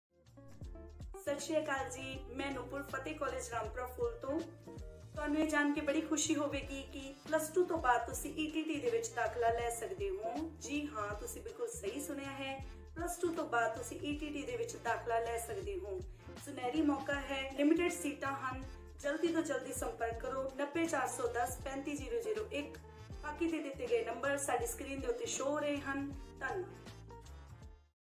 ਕੀ ਤੁਸੀਂ ETT ਕਰਨਾ ਚਾਹੁੰਦੇ ਹੋ ?10+2 ਪਾਸ ਵਿਦਿਆਰਥੀਆਂ ਲਈ ETT ਕਰਨ ਦਾ ਸੁਨਹਿਰੀ ਮੌਕਾ
ਖੁਸ਼ਖਬਰੀ ਖੁਸ਼ਖਬਰੀ ਖੁਸ਼ਖਬਰੀ
ਕੀ ਤੁਸੀਂ  ETT ਕਰਨਾ ਚਾਹੁੰਦੇ ਹੋ ?
10+2 ਪਾਸ   ਵਿਦਿਆਰਥੀਆਂ ਲਈ ETT ਕਰਨ ਦਾ ਸੁਨਹਿਰੀ ਮੌਕਾ
ਜਨਰਲ ਵਿਦਿਆਰਥੀ- 10 + 2 ਦੇ ਨਾਲ 50% ਅੰਕ
ਐਸ.ਸੀ. / ਐਸ.ਟੀ.-10 + 2 ਦੇ ਨਾਲ 45% ਅੰਕ
* ਪੰਜਾਬ ਸਰਕਾਰ/SCERT ਦੁਆਰਾ ਨਿਰਧਾਰਤ ਫੀਸ
* ਕੇਵਲ ਲੜਕੀਆਂ ਲਈ
* ਭਰੋਸੇ ਯੋਗ ਕਾਲਜ ਦੀ ਟਰਾਸਪੋਰਟ ਸੁਵਿਧਾ
* ਸੀਮਤ ਸੀਟਾਂ
ਫਤਿਹ ਗਰੁੱਪ ਆਫ ਇੰਸਟੀਚਿਊਟ, ਮੌੜ ਰੋਡ ਰਾਮਪੁਰਾ ਫੂਲ,ਬਠਿੰਡਾ ਵਿੱਚ ਆਪਣੀਆ ਸੀਟ ਬੁੱਕ ਕਰਵਾਉਣ ਲਈ ਅੱਜ ਹੀ ਸੰਪਰਕ ਕਰੋ-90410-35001,35002,35003,35004